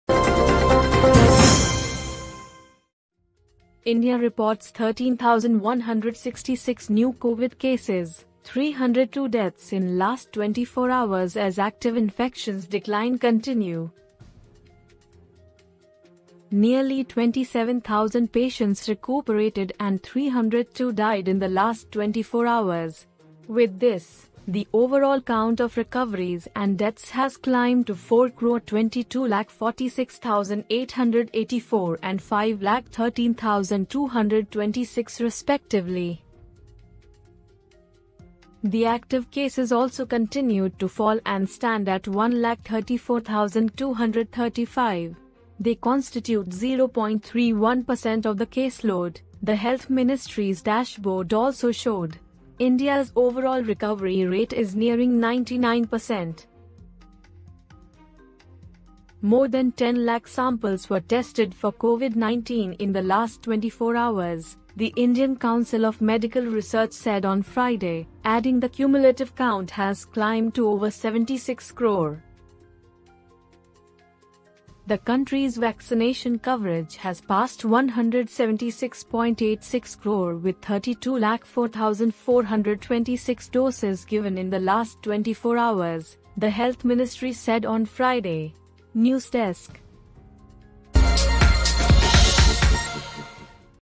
India reports 13,166 new COVID cases, 302 deaths in last 24 hours
India reports 13,166 new Covid cases, 302 deaths in last 24 hours as active infections' decline continue.